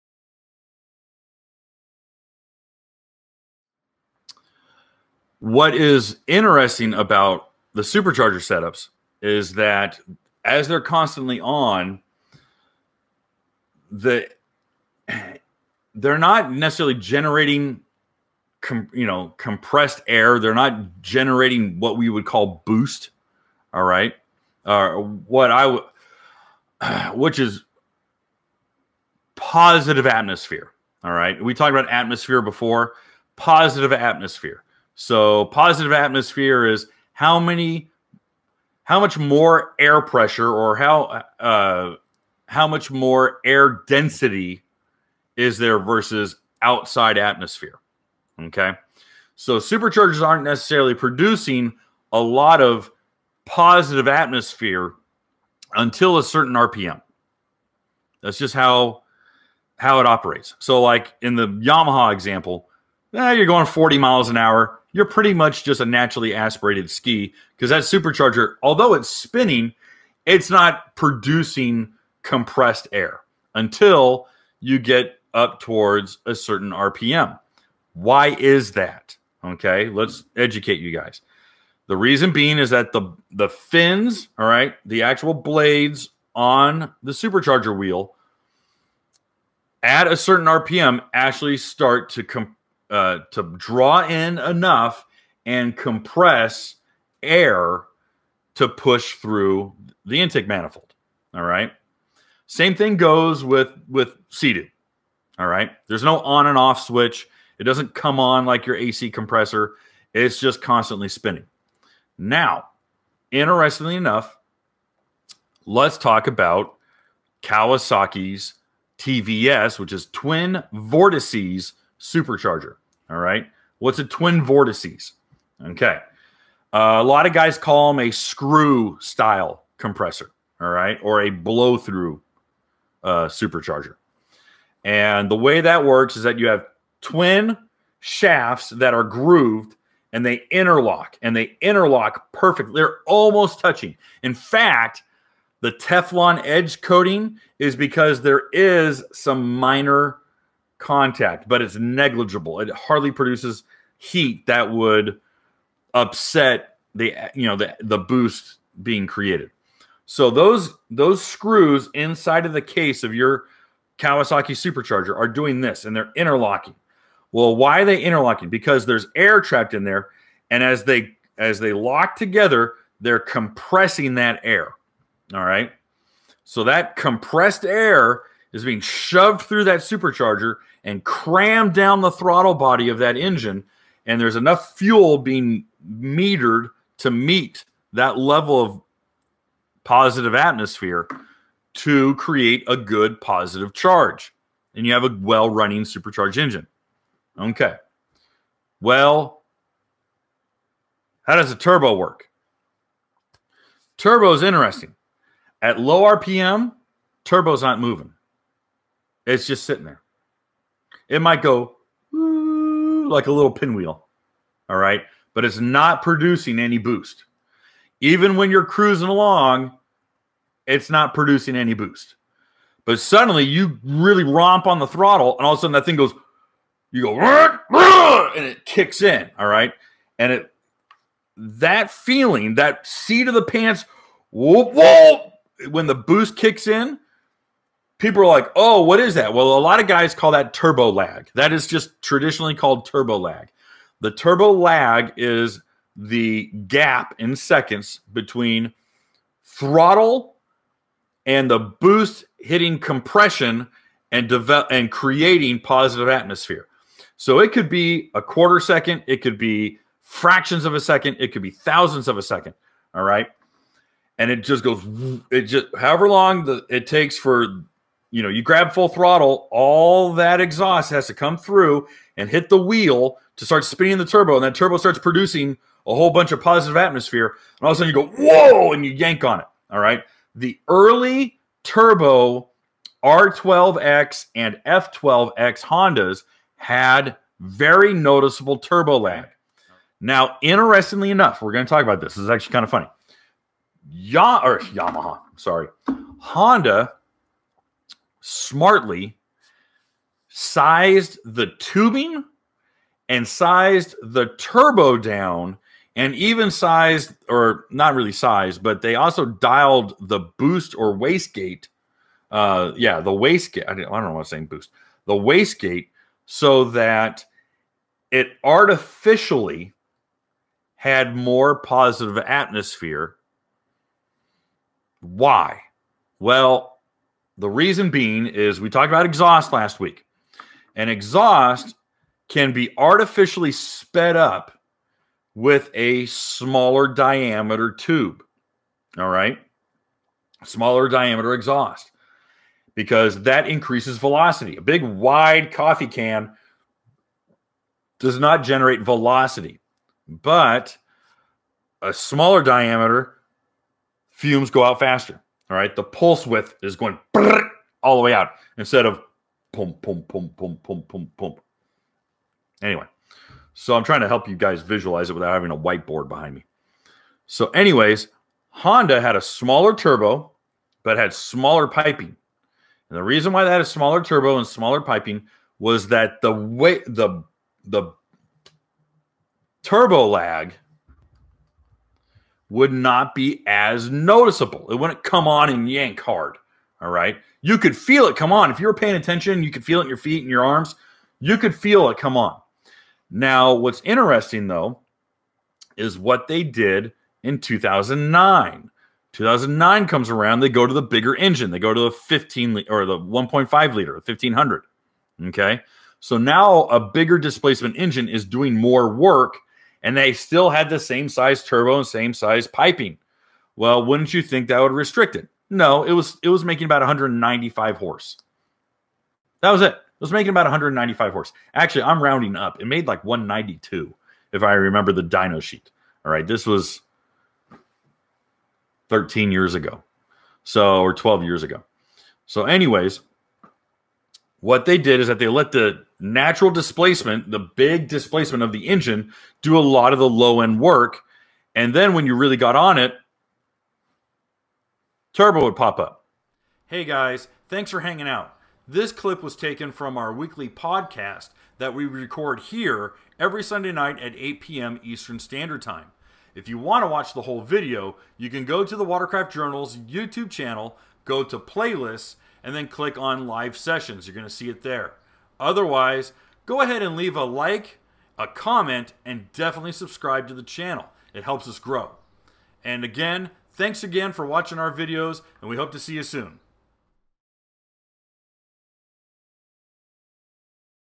Turbos vs. Superchargers: WCJ Quick Topic
This clip was taken from The Watercraft Journal IRL podcast titled, "Will We See a Factory Turbo PWC Again?"